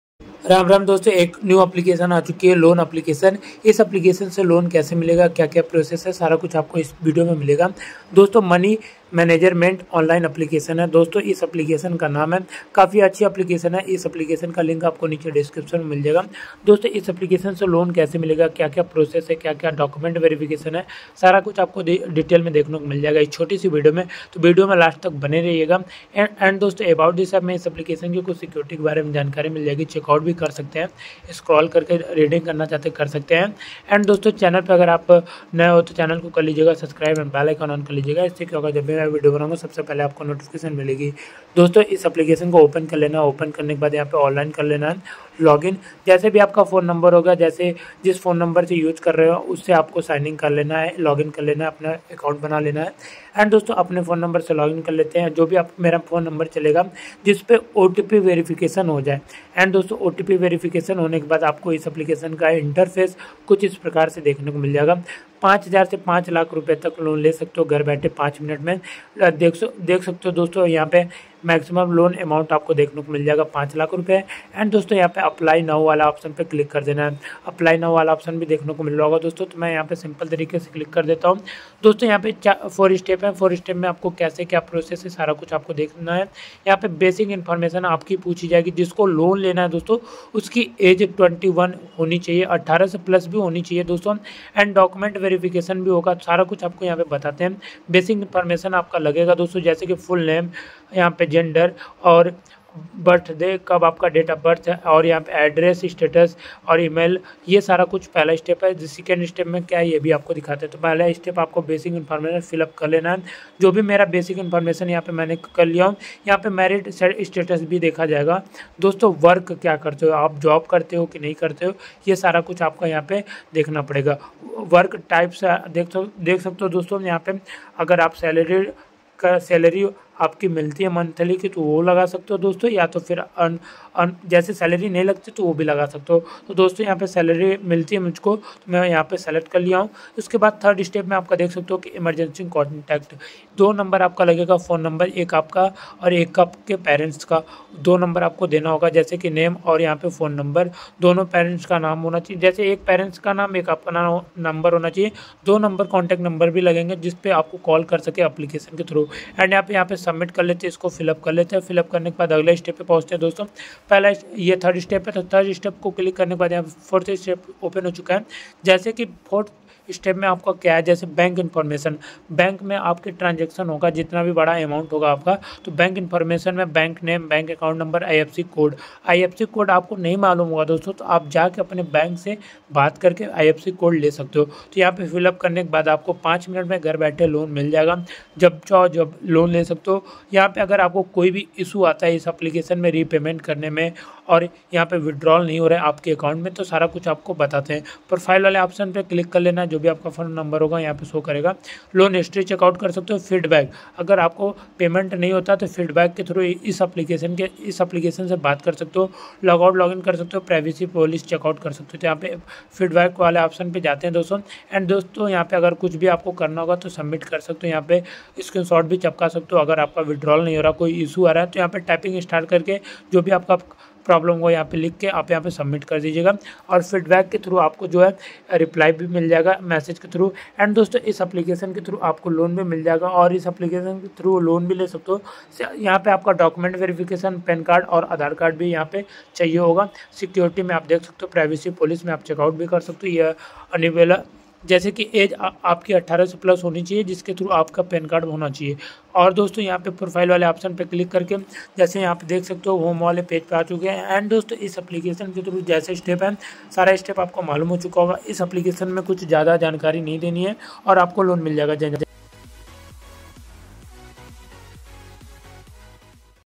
Money Management Online Loan App | Money management online loan app review
Money Manager - the financial planning, review, expense tracking, and personal asset management app for Android Money Manager makes managing personal finances as easy as pie! Now easily record your personal and business financial transactions, generate spending reports, review your daily, weekly and monthly financial data and manage your assets with Money Manager's spending tracker and budget planner.